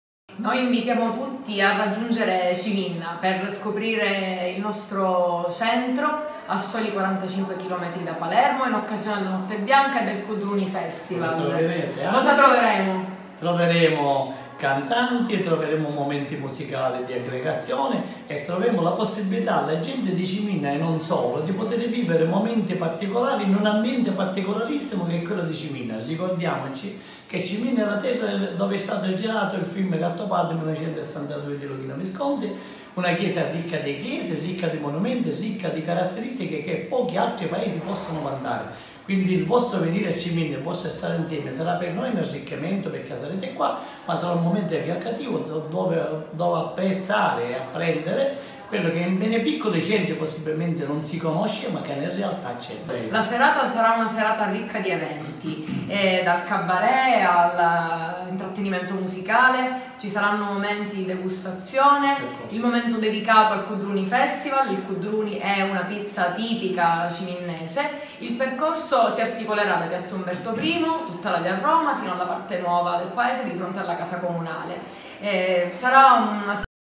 Ciminna: Stasera la Notte bianca e il "Cudduruni Festival"
Cuddiruni festival 2018 e Notte bianca a Ciminna

Tutti pazzi per la focaccia dal sapore forte e deciso, la sagra del "cuddiruni" a Ciminna

Cuddiruni Festival e Notte Bianca 2018 a Ciminna, la manifestazione dedicata al uno dei prodotti tipici del territorio, prenderà il via venerdì 24 agosto. Nell’atmosfera di una Notte Bianca si alterneranno diversi artisti, spettacoli e altro: quest’anno Ciminna ospita il gruppo musicale pugliese “Kardiamundi” per celebrare la Notte della Taranta, suoni, ritmi, danze e musiche originali della Pizzica.

Il "Cuddiruni" è una focaccia dal sapore forte e deciso, panificata come si faceva un tempo e condita con pomodoro, acciughe e origano. L’evento più atteso dell’estate in cui ai sapori tipici della tradizione culinaria ciminnese si uniranno musica e divertimento. Venerdì 24 agosto 2018 sarà possibile degustare la specialità del “cudduruni” nella splendida atmosfera della "Notte Bianca" ricca di artisti e spettacoli.

Il Cuddiruni Festival Ciminna - Rural Day è un evento che valorizza la gastronomica locale ciminnese. Nasce per rafforzare la denominazione di origine del prodotto e rappresenta un’opportunità per aiutare le piccole aziende del territorio per promuovere la produzione locale. E’ un prodotto dei forni e della cucina tipica Ciminnese, una focaccia (un pò più piccola della pizza) di impasto morbido e gustoso, dal sapore forte e deciso, un profumo irresistibile. Viene realizzata bianca (senza la polpa di pomodoro) o rossa, condita con acciughe, cipolle, formaggio pecorino e mollica.